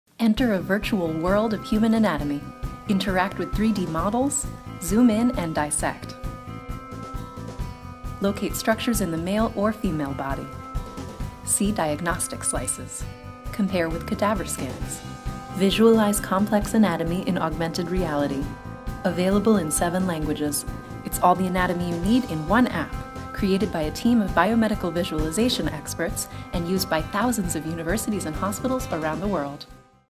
[PROMO] Human Anatomy Atlas 2018 for Android | Visible Body
Human Anatomy Atlas 2018 Edition is the go-to 3D anatomy reference app for healthcare professionals, students, and professors. The app includes comprehensive male and female 3D gross anatomy models, select microanatomy of tissues and organs, cadaver slices and diagnostic images that are paired with 3D cross-sections, and interactive animations of muscles and bones.

Visible Body produces innovative, interactive 3D human anatomy and physiology content and applications. If you love the human body, you'll love us!